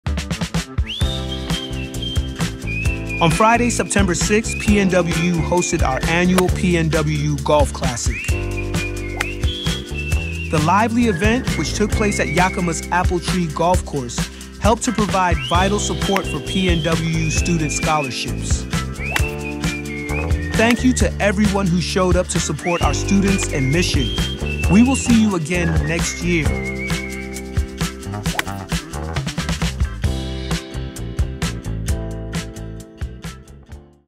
2024 PNWU Golf Classic
On Friday, September 6, PNWU hosted our annual PNWU Golf Classic.

The lively event, which took place at Yakima’s Apple Tree Gold Course, helped to provide vital support for PNWU student scholarships.

Thank you to everyone who showed up to support our students and mission! We will see you again next year!